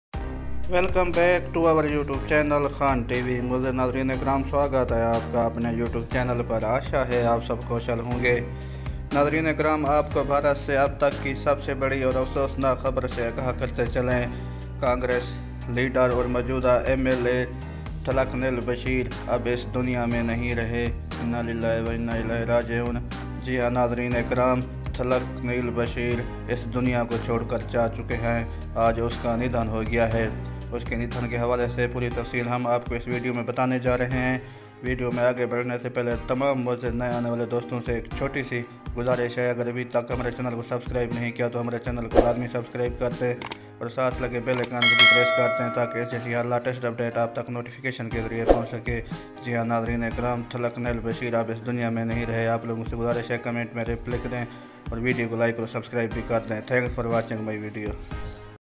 Thalekunnil Basheer Passed Away | "ഒരു ആത്മസുഹൃത്ത് നഷ്ടപെട്ട ദുഃഖമാണ് എനിക്ക്" /Thalekunil Basheer
മുതിര്‍ന്ന കോണ്‍ഗ്രസ് നേതാവ് Thalekkunnil Basheer അന്തരിച്ചു. 79 വയസ്സായിരുന്നു. തിരുവനന്തപുരത്തെ വെമ്പായത്തെ വീട്ടിൽ വെച്ച് പുലര്‍ച്ചെ നാലരയോടെയായിരുന്നു അന്ത്യം. ഹൃദയ സംബന്ധമായ അസുഖങ്ങളെ തുടർന്ന് അഞ്ച് വർഷത്തോളമായി സജീവ രാഷ്ട്രീയത്തില്‍ നിന്ന് മാറി വിശ്രമജീവിതത്തില്‍ ആയിരുന്നു.